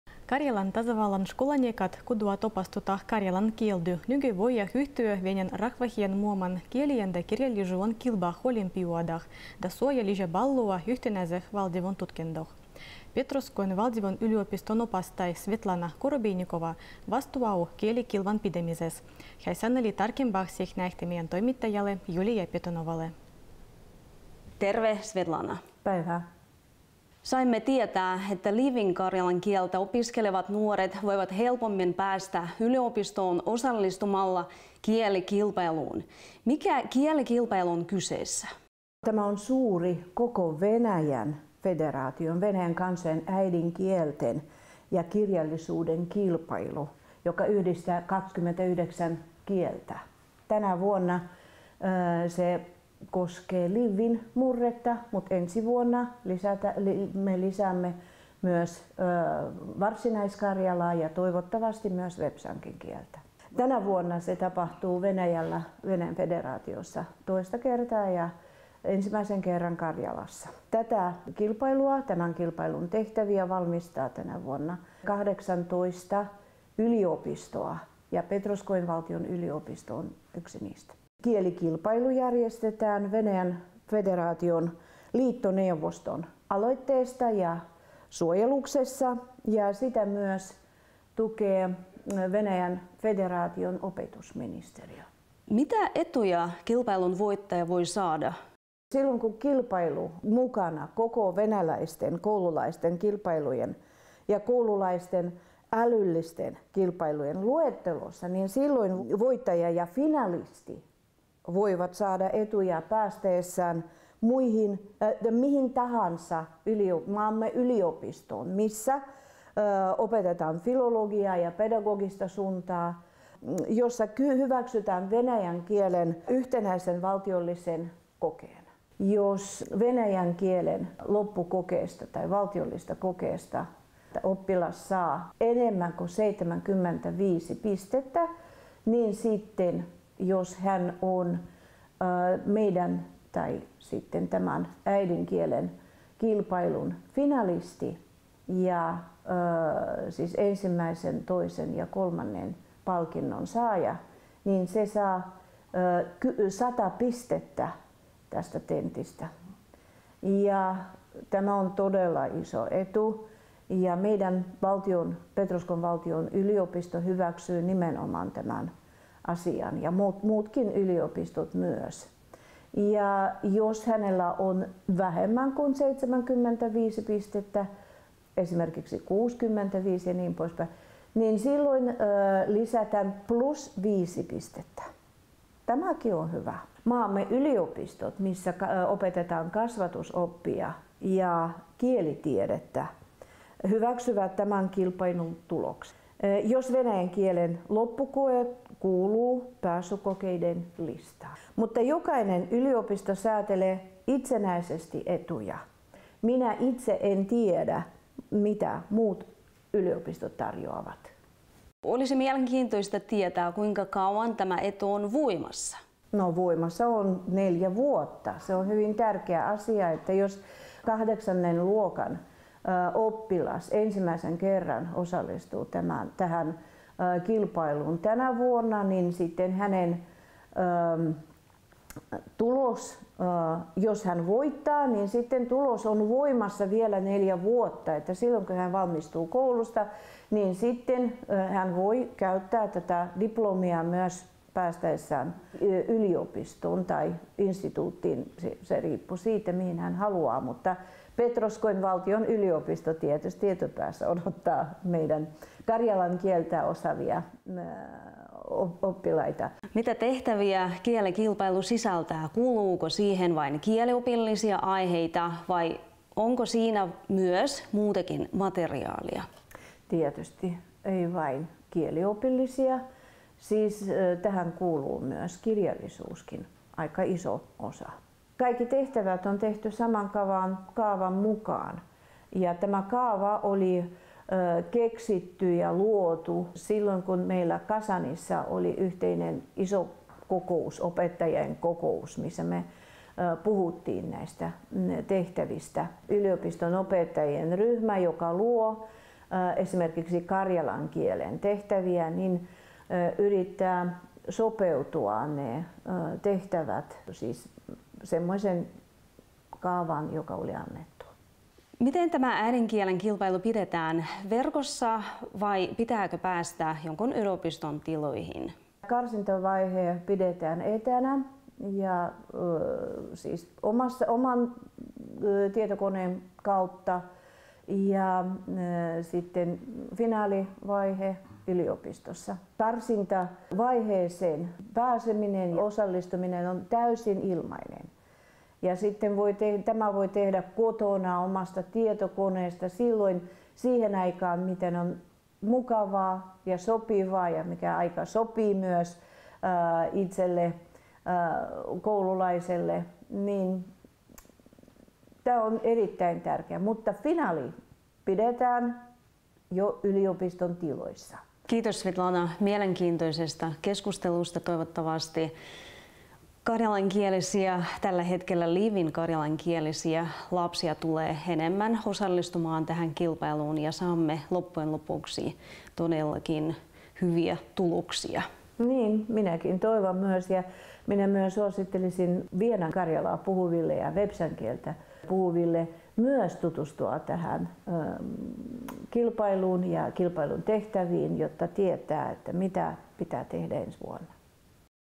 Haku äidinkielten ja kirjallisuuden kilpailuun jatkuu
Karjalan kieltä opiskelevat koululaiset saivat ainutlaatuisen mahdollisuuden osallistua Venäjän kansojen äidinkielten ja kirjallisuuden kilpailuun ja saada lisäpisteet valtion yhteinäiseen tenttiin. Tarkemmin asiasta meille kertoi Petroskoin valtionyliopiston opettaja Svetlana Korobeinikova, joka koordinoi tätä kilpailua tasavallassame.